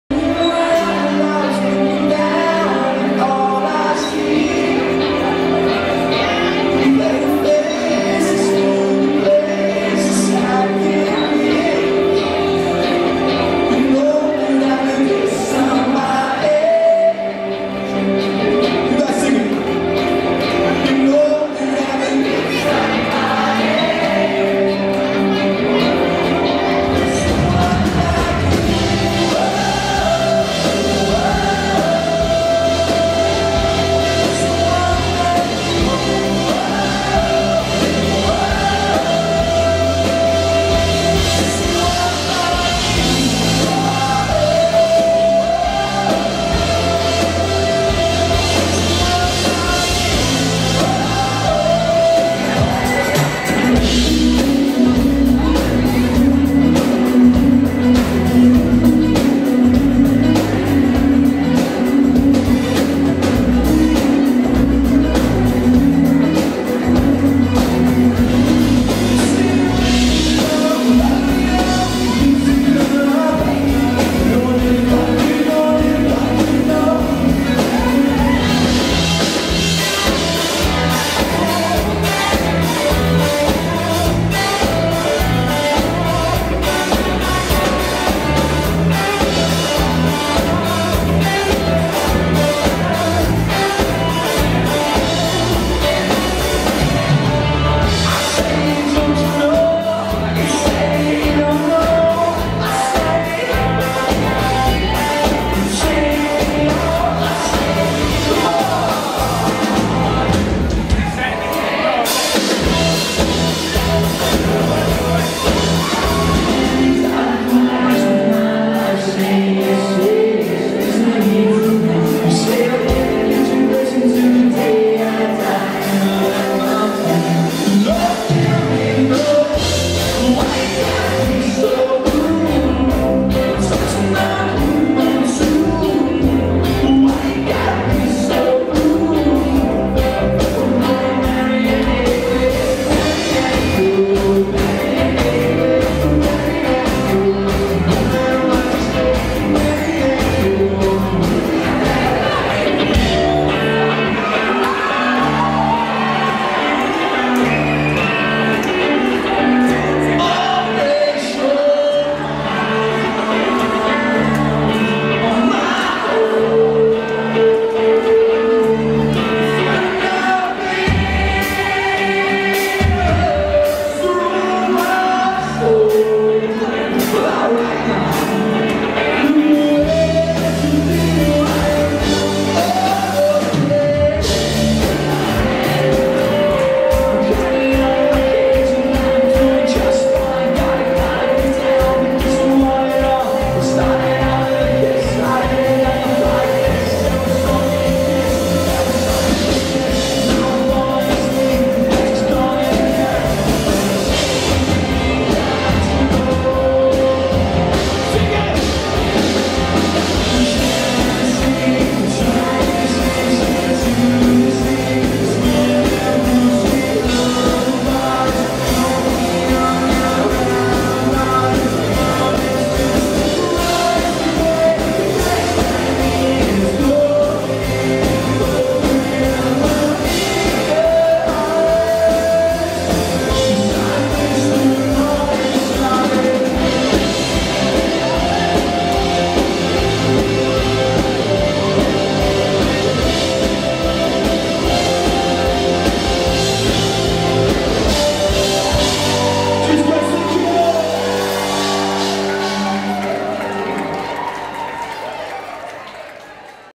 Indie / Rock Band | Kaiser Monkey Killers | Live | Big Foot Events
Introducing The Kaiser Monkey Killers, THE best indie rock & Brit pop tribute band! This is REAL live music, live, gritty and loud! They sound uncannily like the artists they cover and don’t just play the songs but deliver them! This is performance, this is indie rock ‘n’ roll, this is the KAISER MONKEY KILLERS!